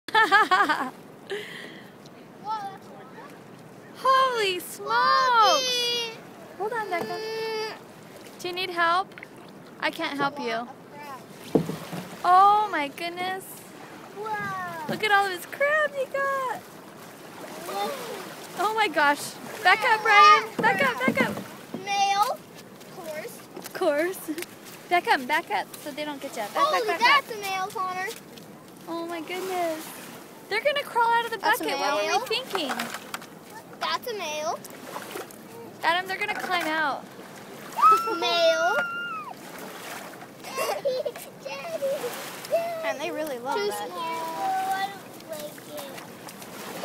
Oregon Kayak crabbing! Netarts Bay
Our first time successfully crabbing. This was the first pull EVER. I didn't have a bucket so I paddled back to shore. It was the first week of the season in August. We took home a TON that first day. A great time if you are going for it, on a kayak is a rush!